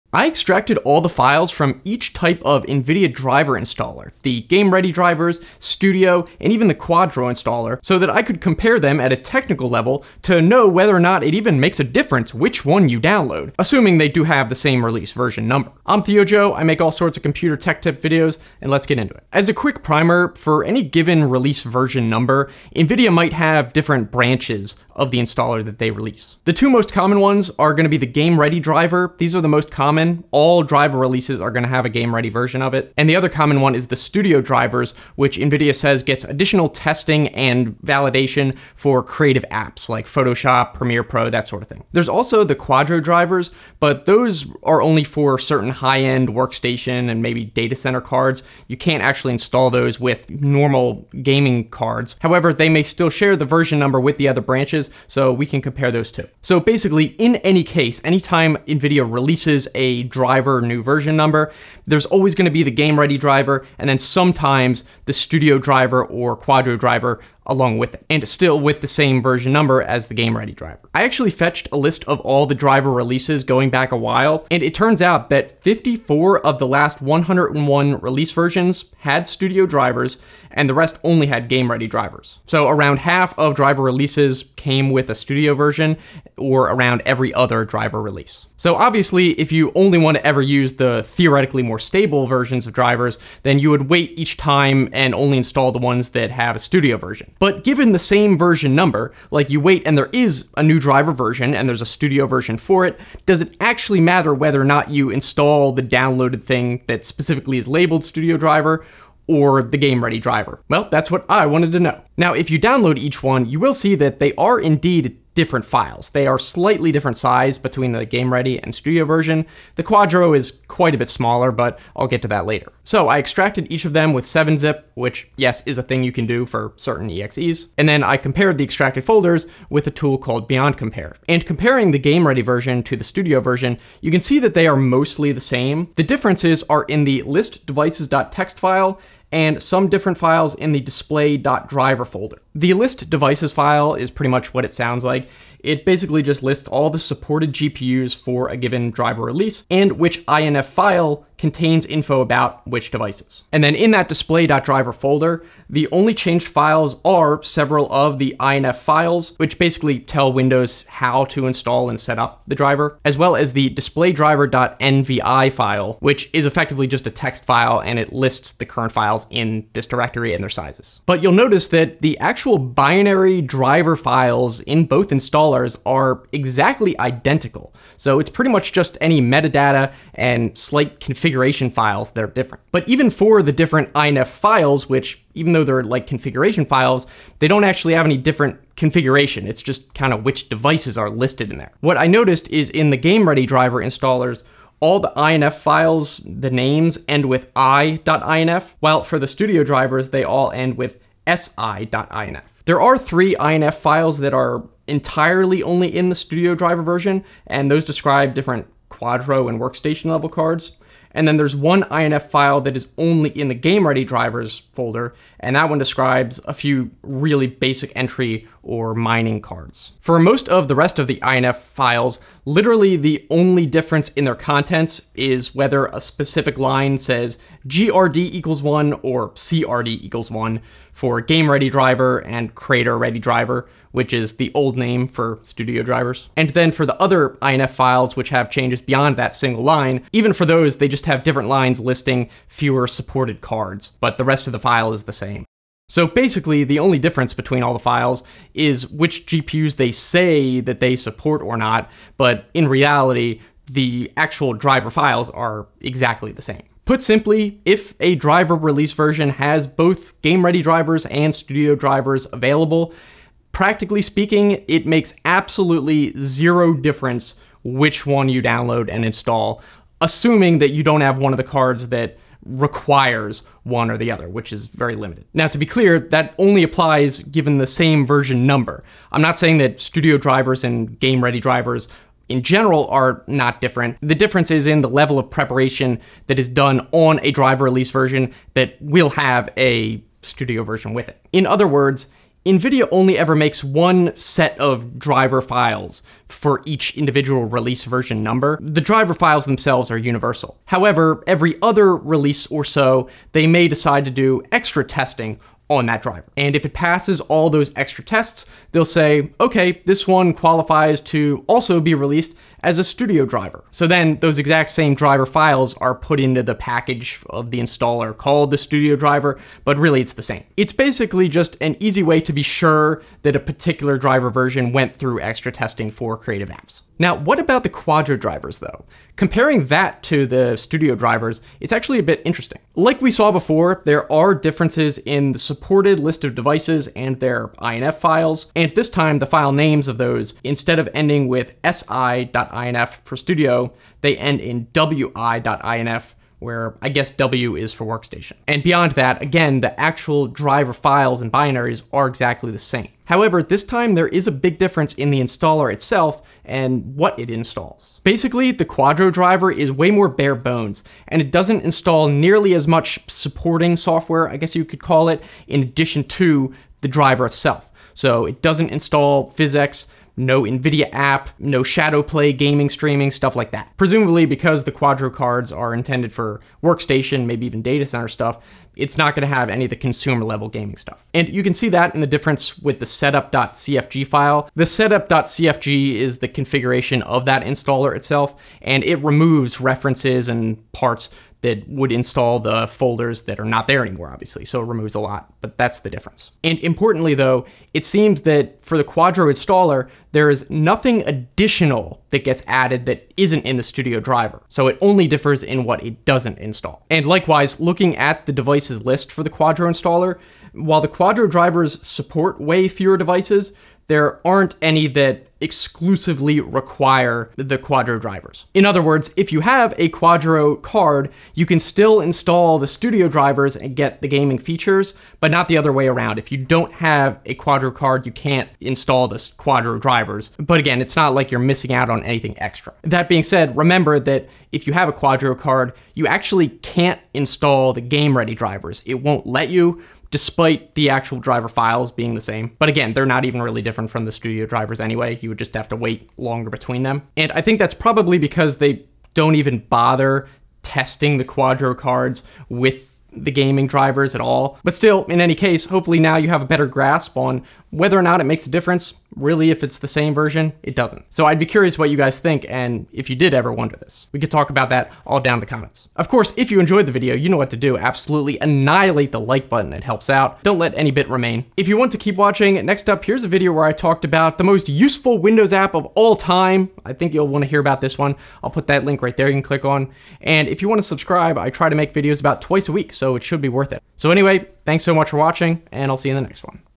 What If You Install The "Wrong" NVIDIA Driver?
Do the different Nvidia driver versions actually make a difference? 🤔

When you go to download the latest Nvidia driver for your GPU, you sometimes see multiple types such as the Game-Ready drivers, Studio Drivers, or even Quadro drivers. But does it actually matter about which one you download, assuming the version number is the same? I wanted to find out, so I downloaded and extracted each of the installers to inspect the actual file differences to know for sure. So next time you won't have to wonder about which version to install for your graphics card.

Time Stamps:
0:00 - Intro
0:19 - Release Branches
1:37 - Comparing The Installers
3:02 - Technical Differences
4:05 - The Difference in Practice
5:26 - What About Quadro Drivers?